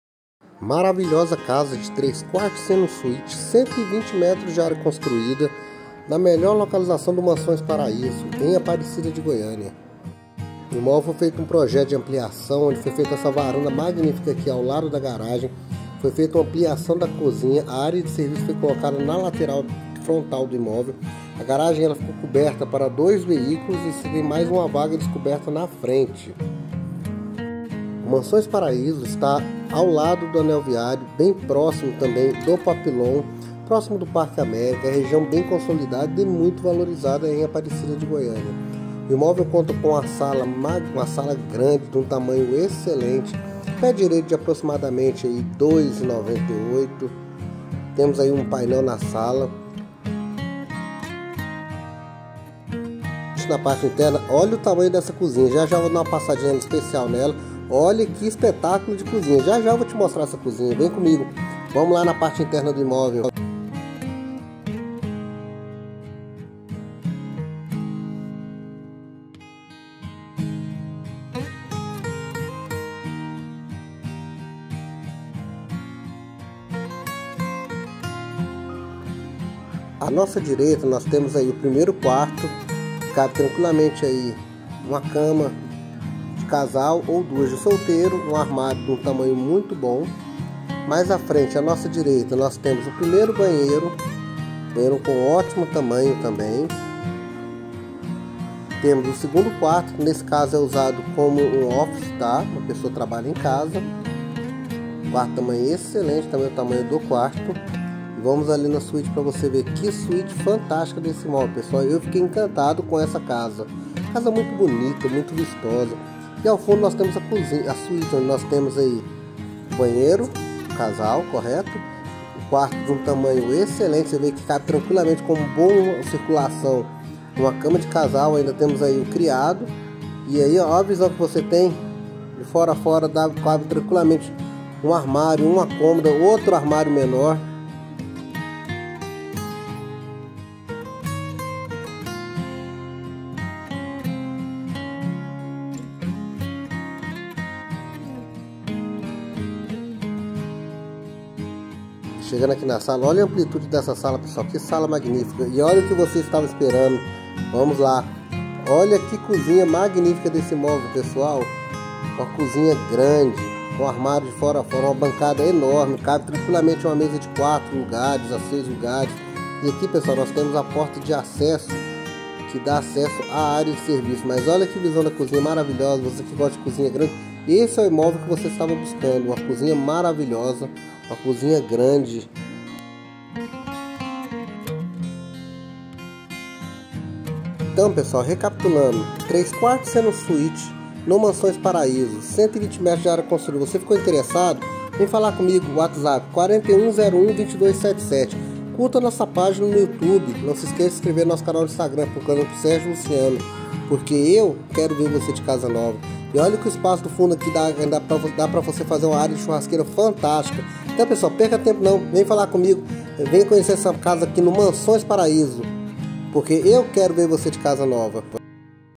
linda casa de 3/4 c suite no Mansões Paraíso em Aparecida de Goiânia
Casa 3/4 c suite 120 m de área construída e 180 de lote, setor Papillon park ao lado do Anel Viário e perto do comércio R$ 275.000,00

Casa com excelente acabamento e ótima localização

 Agende hoje mesmo sua visita

Sérgio Luciano
CRECI 18246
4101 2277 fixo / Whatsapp